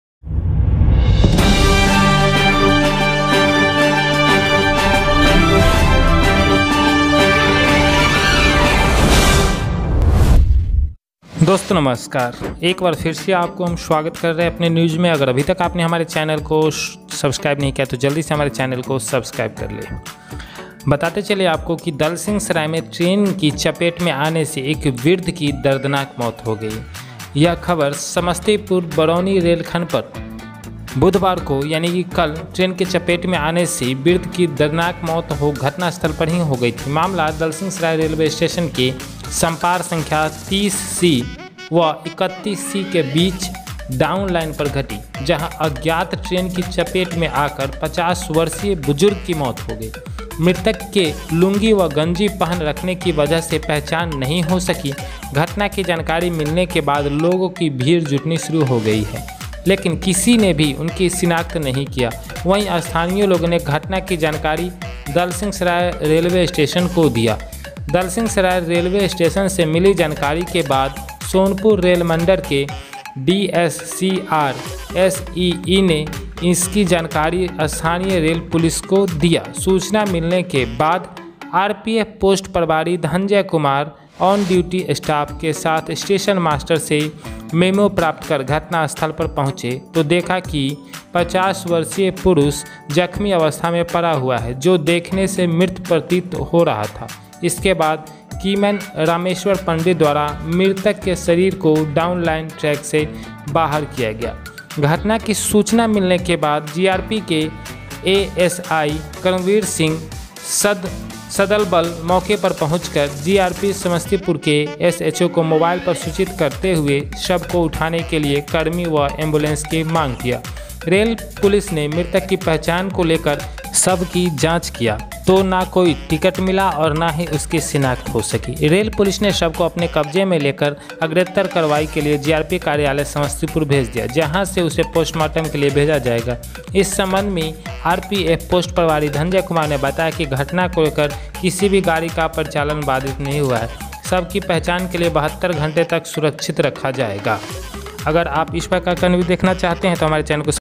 दलसिंहसराय में ट्रेन से कटकर बृद्ध की मौत।dalsinghsaray me tren se katkar vridh ki maut
दलसिंहसराय में ट्रेन से कटकर बृद्ध की मौत
 dalsinghsaray me tren se katkar vridh ki maut
 abb news
abbnews
news4abb
ashish aryan
samastipur news
 train
train durghatna